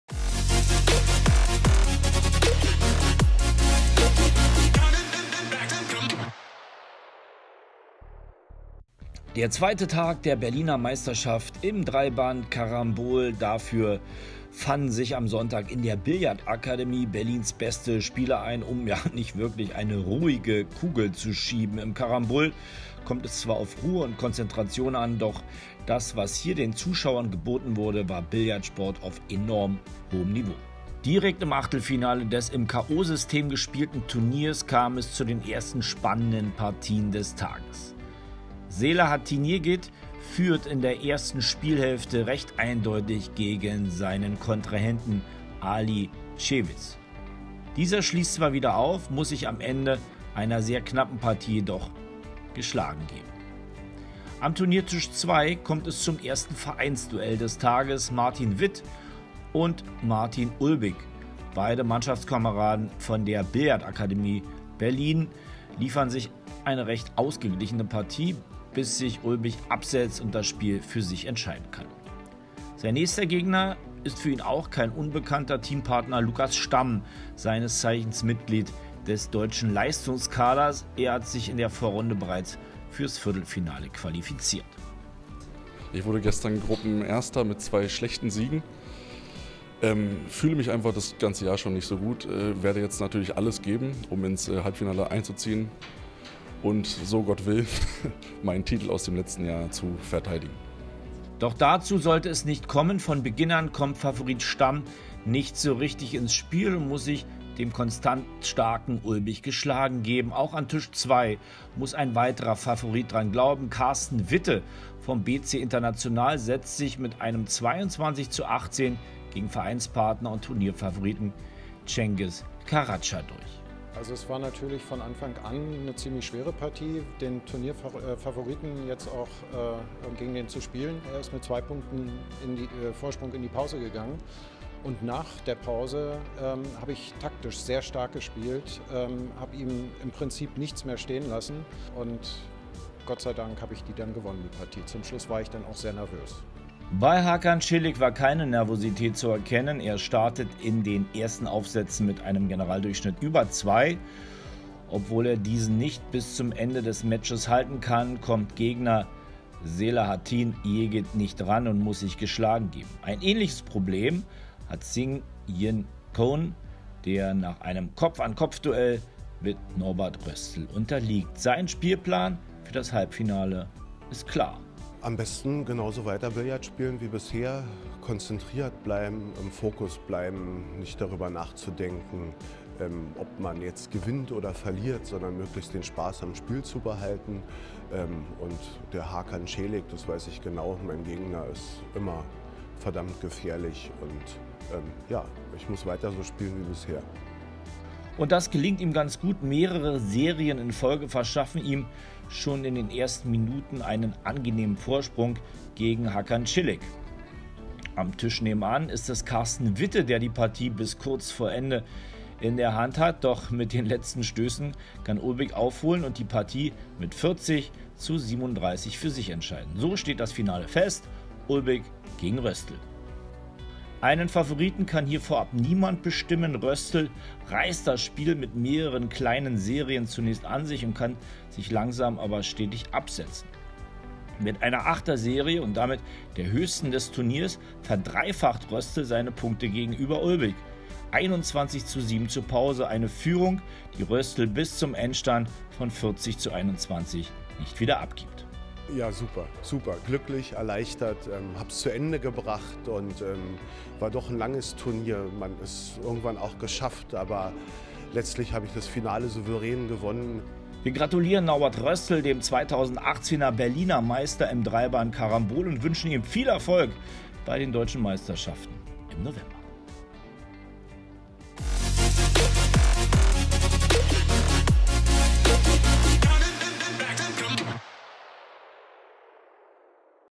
Berliner Meisterschaften 2018 der Herren im Dreiband Karambol Billard
Es ist der zweite Tag der Berliner Meisterschaft im 3-Band Karambol. Dafür fanden sich am Sonntag in der Billardakademie, Berlins beste Spieler ein um - ja nicht wirklich eine ruhige Kugel zu schieben.

Im Karambol kommt es zwar auf Ruhe und Konzentration an, doch das was hier den Zuschauern geboten wurde, war Billard-Sport auf enorm hohem Niveau.